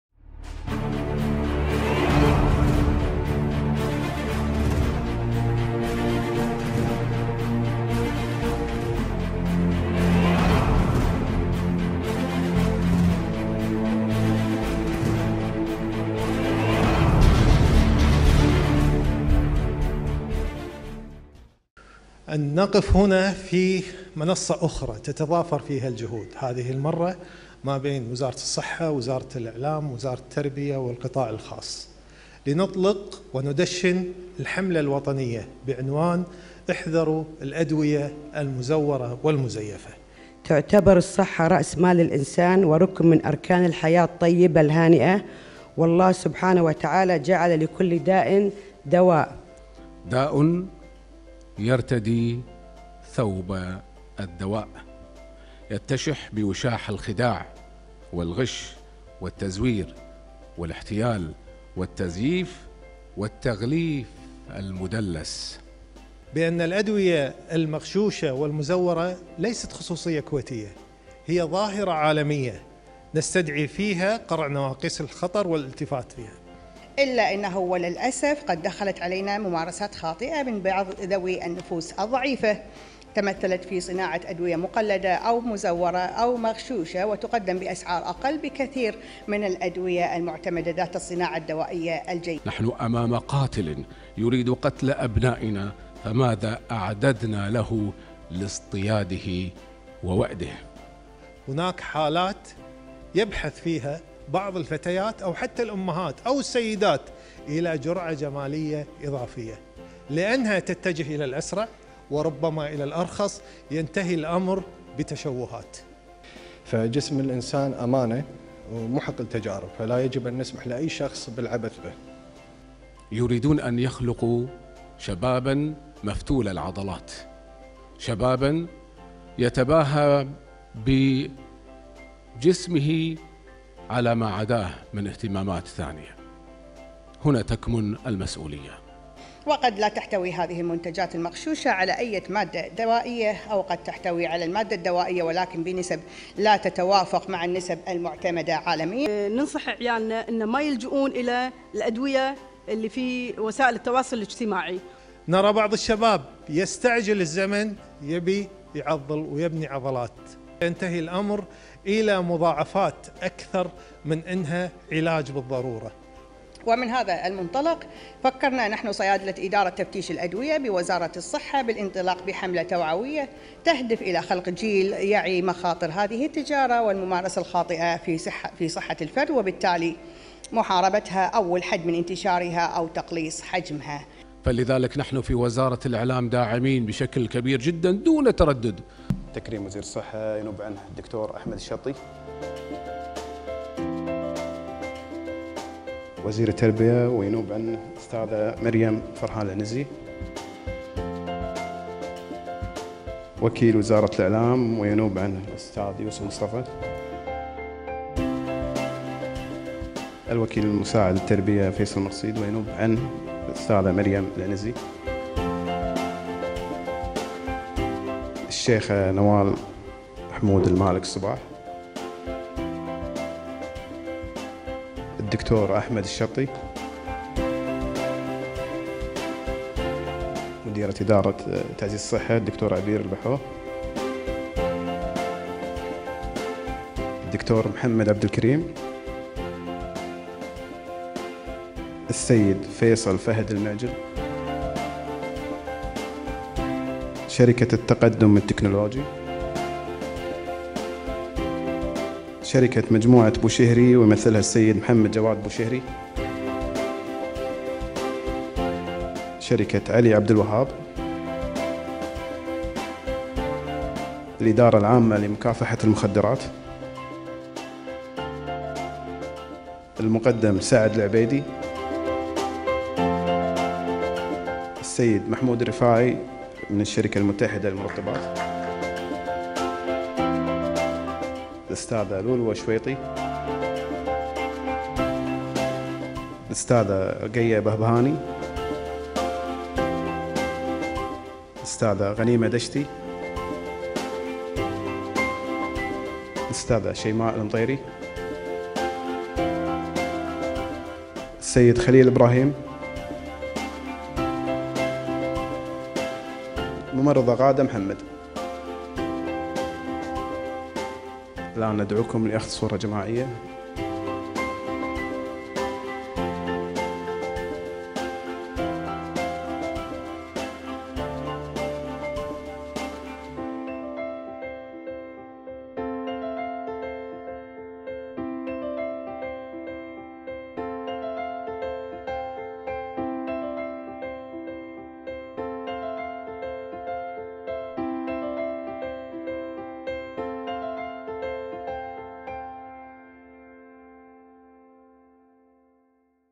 إحذروا الأدوية المزيفة والمزورة
«الصحة»: احذروا الأدوية المزيفة

31 أكتوبر، 2017

كونا – أكد المتحدث الرسمي لوزارة الصحة الدكتور احمد الشطي اليوم الثلاثاء اهمية الحملة الوطنية المشتركة «احذروا الادوية المزيفة والمزورة» في توعية طلبة المدارس من مخاطر هذه الظاهرة.
جاء ذلك في تصريح صحفي للاعلان عن الحملة التي تتبنها «الصحة» ممثلة في ادارة تفتيش الادوية بالتعاون مع وزارة التربية ممثلة في قطاع التنمية التربوية والانشطة والمقرر تنظيمها في الفترة من 1 نوفمبر المقبل وحتى 25 ابريل 2018.
وأوضح الشطي ان فعاليات الحملة ستشمل اقامة محاضرات علمية وانشطة توعوية متنوعة يستفيد منها طلبة وطالبات 36 مدرسة ثانوية من مختلف المناطق التعليمية بغية توعيتهم من مخاطر الادوية والمستحضرات والهرمونات البشرية والحيوانية المزيفة.
وأضاف ان الغش لم يعد مقتصرا على السلع الاستهلاكية انما طال صحة الافراد من خلال التلاعب في الادوية ما يؤثر على صحتهم وصولا الى الوفاة.
وذكر ان حجم التجارة العالمية للادوية يناهز 800 مليار دولار امريكي تصل نسبة الادوية المزيفة فيها الى 8 في المئة اي ما يعادل 75 مليار دولار وذلك “وفقا لبعض التقديرات”.
واشار الشطي الى ان “ظاهرة الادوية المزيفة ترتفع في الدول النامية لتبلغ نحو 40 في المئة من حجم تجارتها” مؤكدا انها “انتشرت بسرعة كبيرة في السنوات الاخيرة وبحجم كبير لتطول معظم الادوية والمستحضرات الصيدلانية”.
من جانبها قالت رئيسة الاتحاد العربي لمكافحة التزوير والتزييف الشيخة نوال حمود المالك الصباح في تصريح مماثل ان «الاتحاد العربي» يعد من اهم الاتحادات المتخصصة في مجال مكافحة الغش بالصناعات الدوائية.
واضافت الشيخة نوال ان مثل هذه الحملات بما تتضمنه من رسائل وتوجيهات وارشادات تسهم في زيادة الوعي المجتمعي من انتشار الادوية المزيفة.
وشددت على ضرورة “سن قوانين وتشريعات تتعامل مع غش الادوية كجريمة انسانية ووطنية” مؤكدة اهمية “فرض عقوبات رادعة بحق المزورين”.
من جهتها حذرت مديرة ادراة تفتيش الادوية بوزارة الصحة الدكتورة مروى الجسار في تصريح من خطورة الشراء العشوائي للادوية غير المصرح ببيعها او تداولها تجنبا للمضاعفات الخطيرة التي قد تخلفها على صحة الانسان.
بدورها قالت ممثلة الوكيل المساعد للتنمية التربوية والانشطة بوزارة التربية مريم العنزي في تصريح مماثل ان «التربية» تولي الجانب الصحي لدى طلبة المدارس اهتماما بالغا لاسيما ان “العلم المعرفي والمعلومات الاكاديمية لم تعد كافية لتحصين الانسان من الافات المجتمعية”.